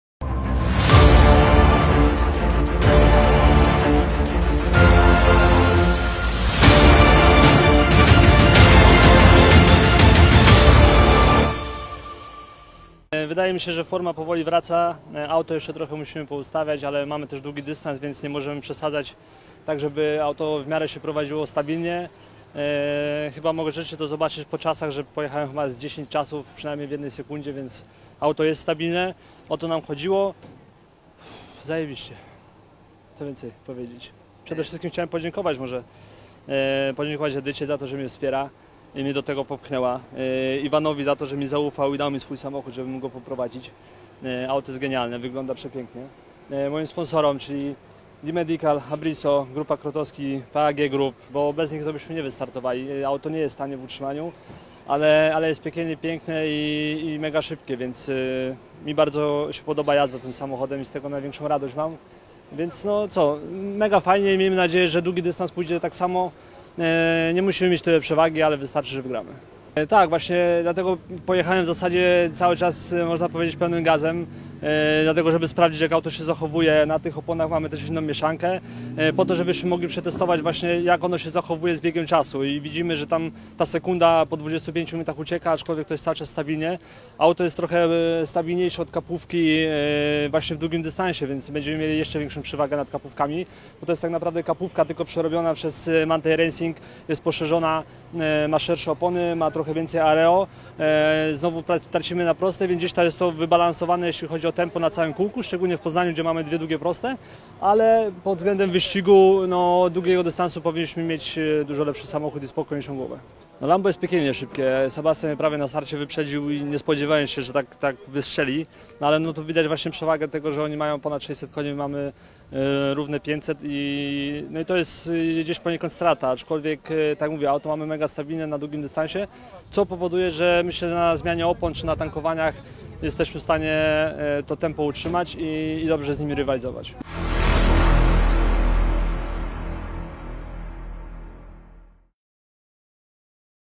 Maciej Bernacik po 3. rundzie Wyścigowych Samochodowych Mistrzostw Polski - WSMP 2021
Maciej Bernacik wygrał sobotni wyścig w D4 +3500 i był też najszybszym kierowcą w Porsche. Opowiada o odbudowywaniu formy po przerwie, rywalizacji w długim dystansie i porównuje swoje auto do Lamborghini Huracana i Radicala SR8